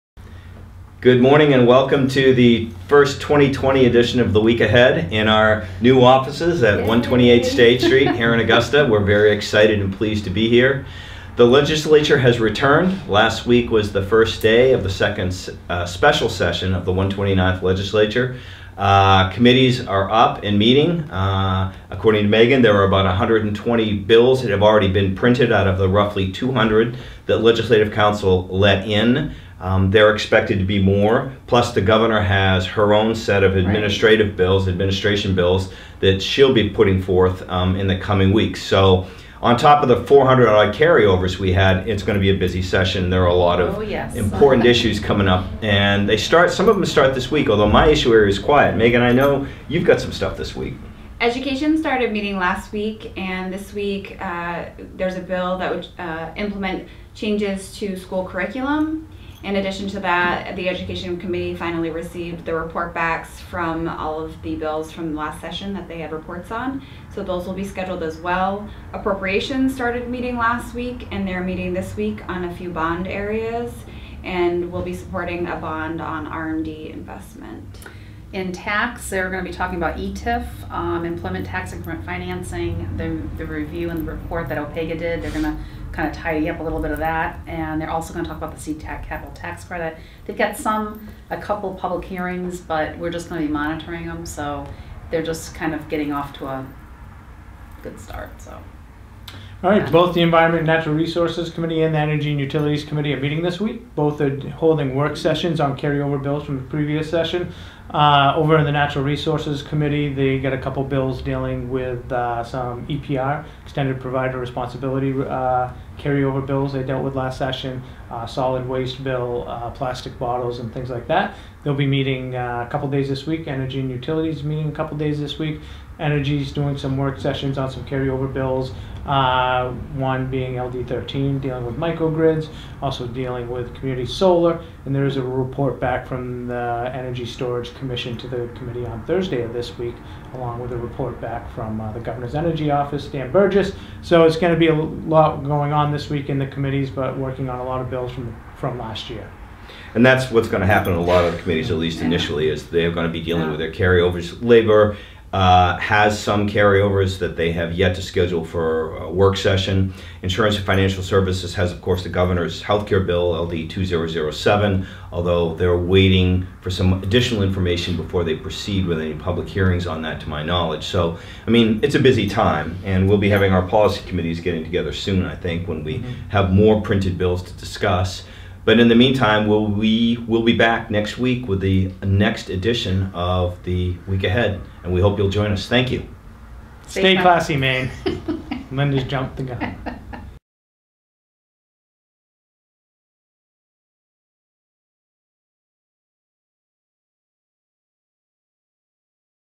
Maine State Chamber's Legislative Week Ahead | January 13, 2020
Peter Gore, Ben Gilman, Megan Diver, Linda Caprara